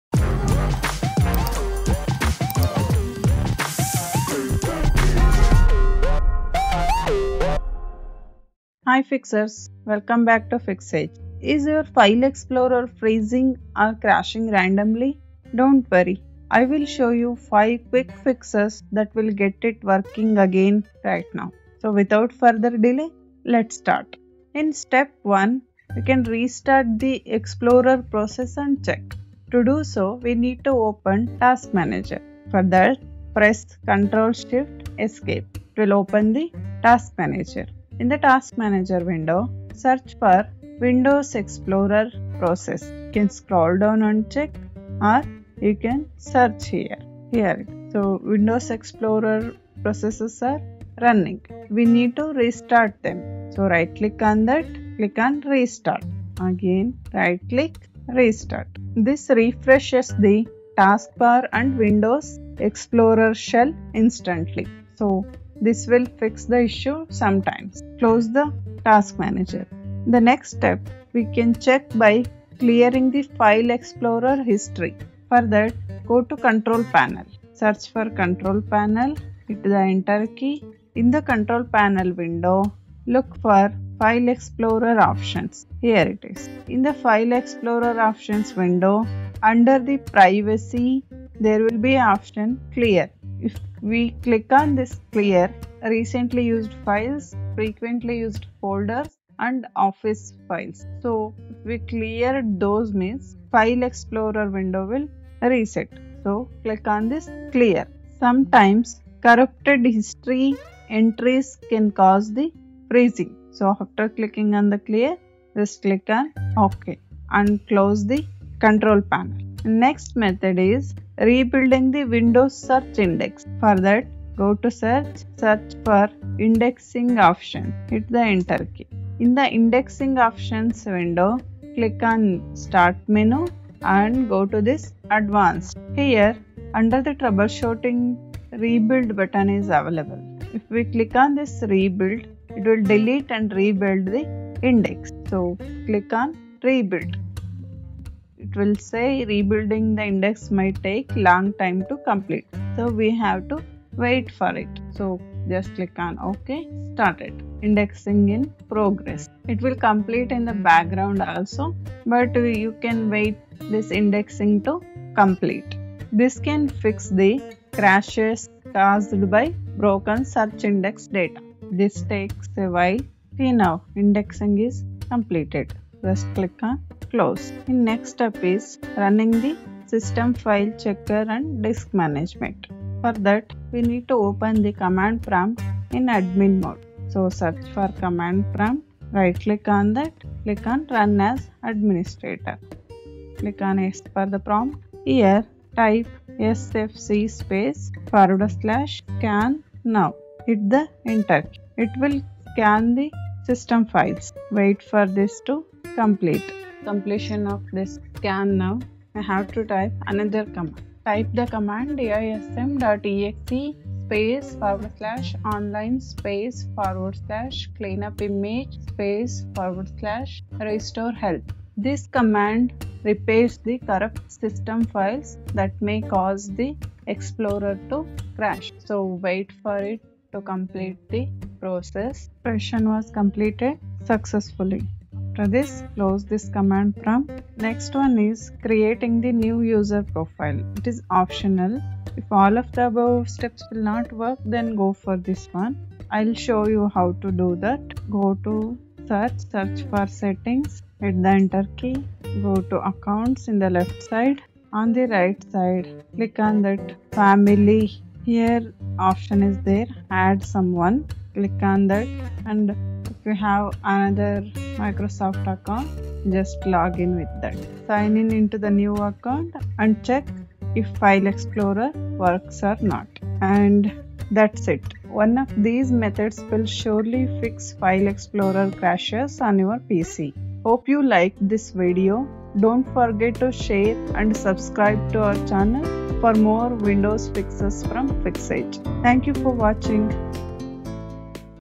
Fix File Explorer Freeze & Crash – Win 10/11
Is File Explorer freezing, not responding, or crashing on your Windows 10 or Windows 11 PC?
In this step-by-step guide, I’ll show you quick and effective methods to get File Explorer working smoothly again.

🛠 What You'll Learn:
• Restart File Explorer from Task Manager
• Clear File Explorer history to remove corrupted cache
• Disable Quick Access to prevent slow loading
• Check for Windows updates that fix stability issues
• Run SFC & DISM commands to repair system files